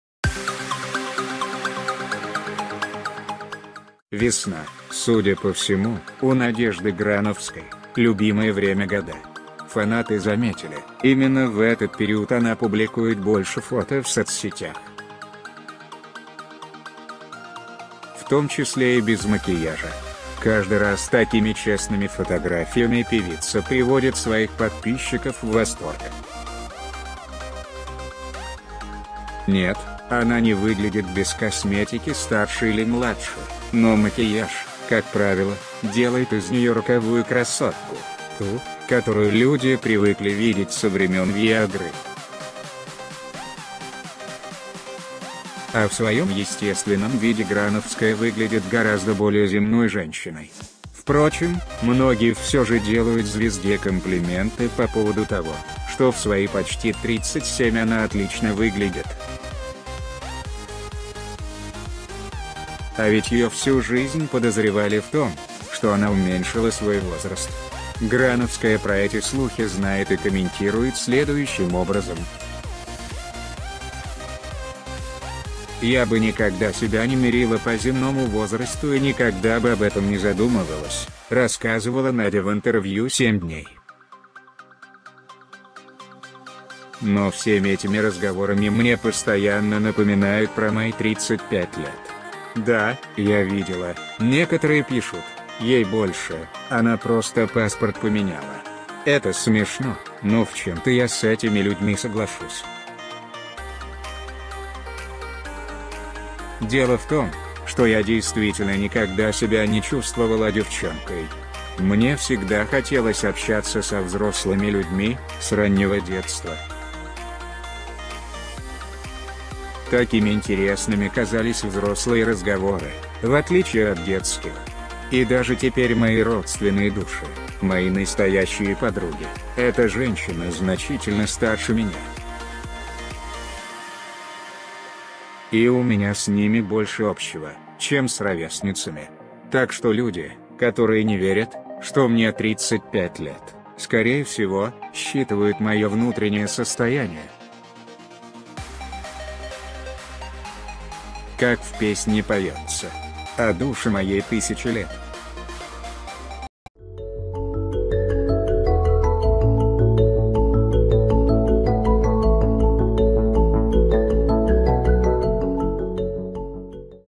Надежда Грановская без макияжа удивила фанатов - Sudo News
Весна, судя по всему, у Надежды Грановской - любимое время года. В том числе и без макияжа. Нет, он

без макияжа, Надежда Грановская,